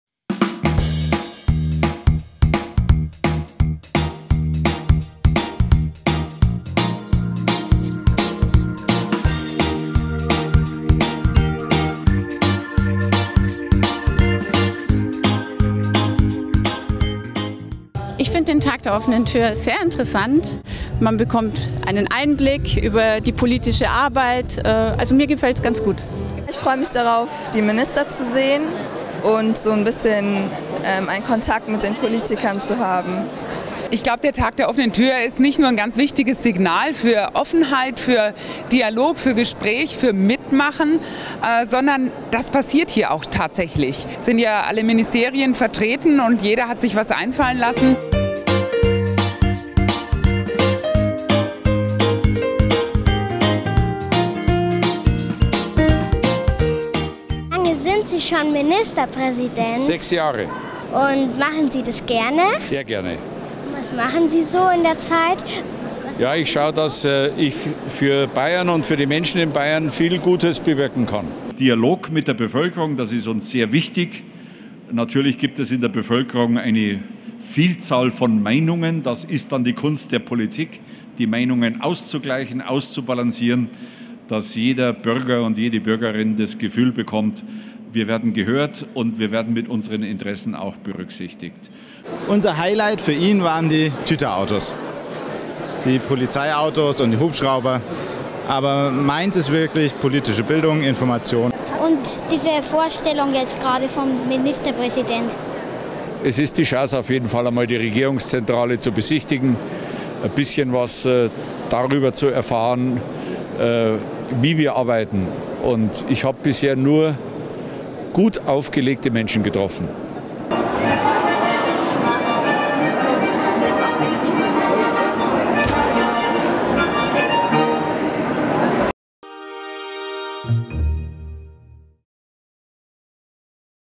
Tag der offenen Tür in der Bayerischen Staatskanzlei - Bayern
Die Türen sind offen und die Besucher strömen durch die Staatskanzlei. Hier findet ihr Eindrücke vom heutigen Tag der offenen Tür.